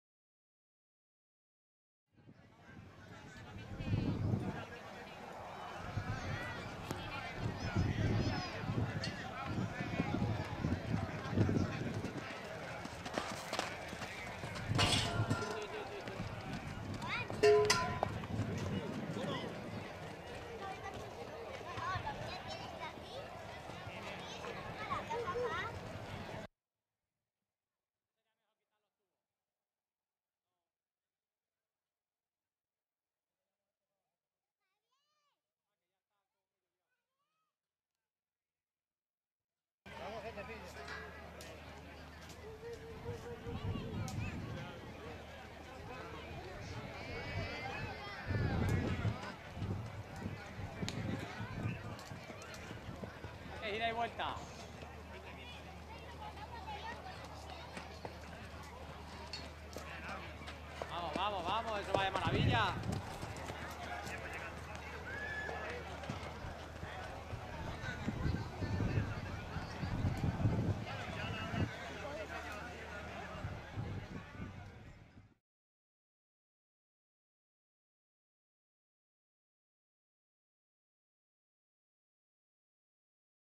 II Grand Prix Caballos Gymkana 2 Lagartera (Toledo)
II Grand Prix Carchena en Septiembre de 2015 en los alrededores de la Ermita de los “Santos Mártires” en Lagartera (Toledo)

Gymkana caballos de deporte